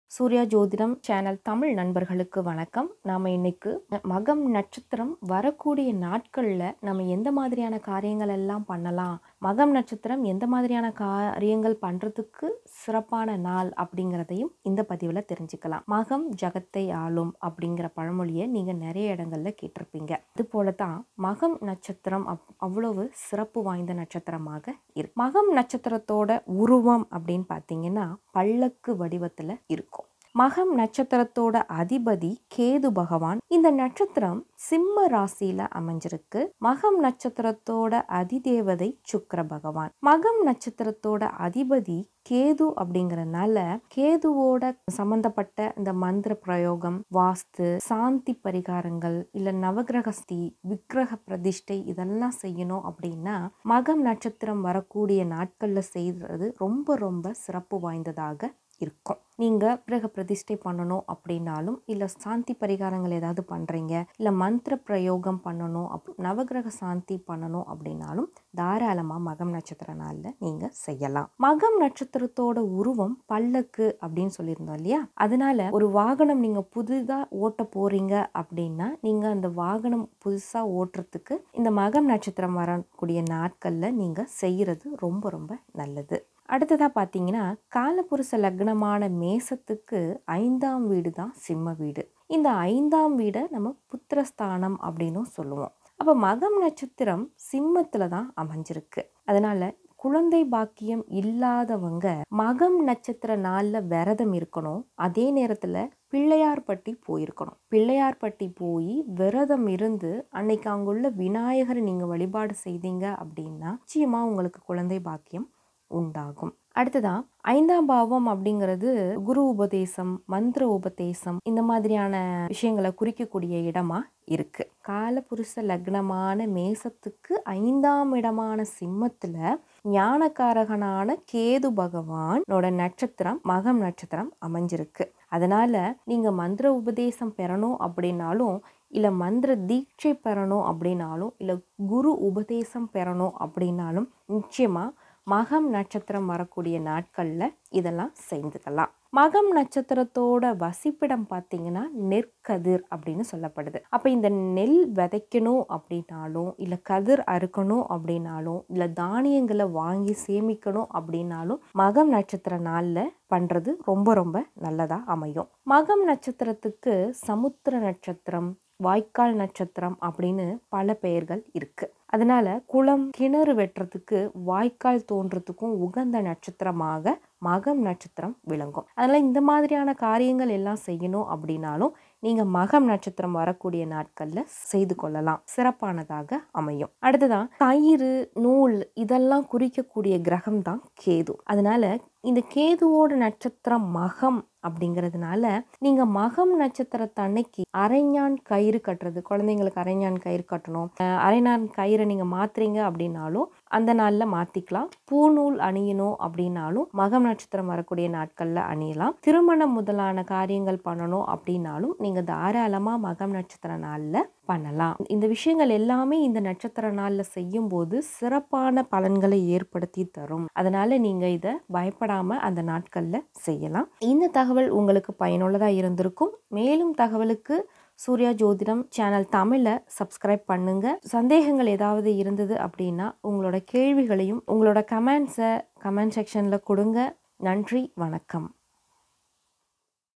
மகம் ||Magam நட்சத்திரம் வரும் நாளில் என்னென்ன செய்யலாம்
மகம் நட்சத்திரம் வரும் நாளில் என்னென்ன விஷேசமான காரியங்கள் செய்யலாம் என்பது பற்றி பதிவிடப்பட்டுள்ளது.

குழந்தைபாக்கியம் பெற மகம் நட்சத்திர நாளில் செய்ய வேண்டியது என்ன ?

magam natchathiram
ganapathy
vinayagar
pillaiyar